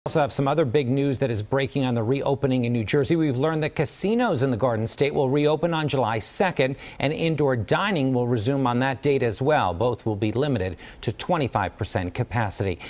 New Jersey Indoor Dining, Casinos Can Reopen July 2
Both will be limited to 25% capacity.